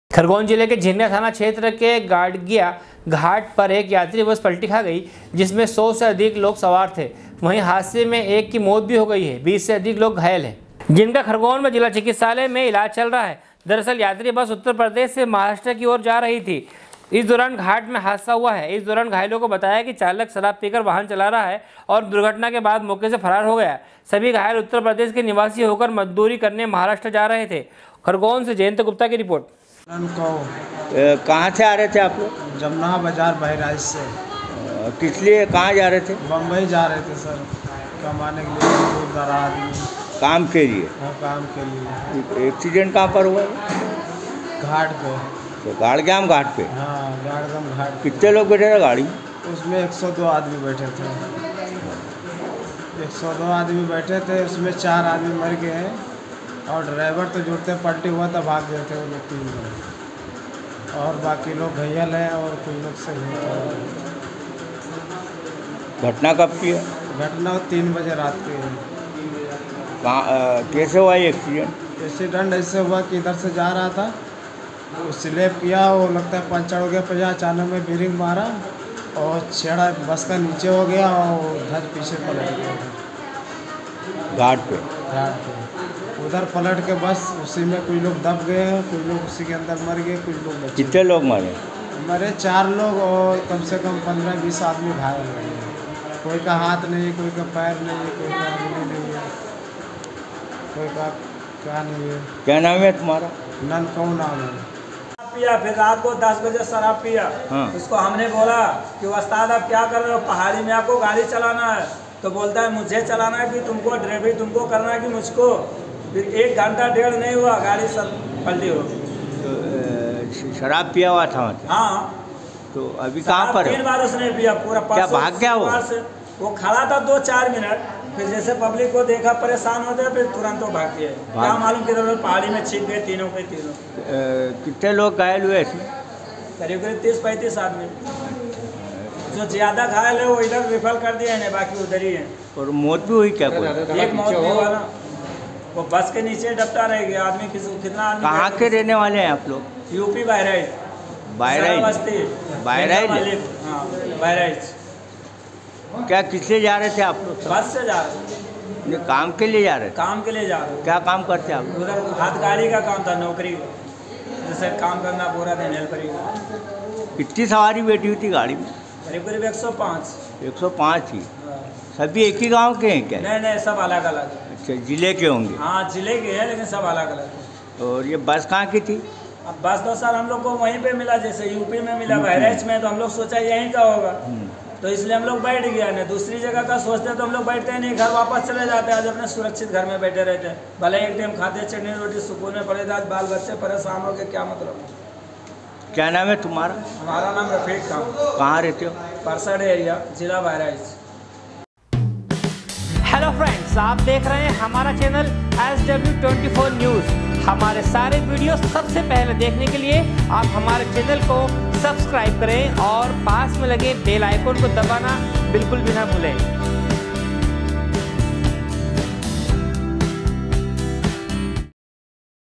khargone- खरगोन जिले के झिरन्या थाना क्षेत्र के गाड़ग्याआम घाट पर एक यात्री बस पलट गयी
खरगोन जिले के झिरन्या थाना क्षेत्र के गाड़ग्याआम घाट पर एक यात्री बस पलट गयी जिसमे सौ से अधिक लोग सवार थे वही हादसे में एक की मौत और बीस से अधिक लोग घायल है । खरगोन से जयंत गुप्ता की रिपोर्ट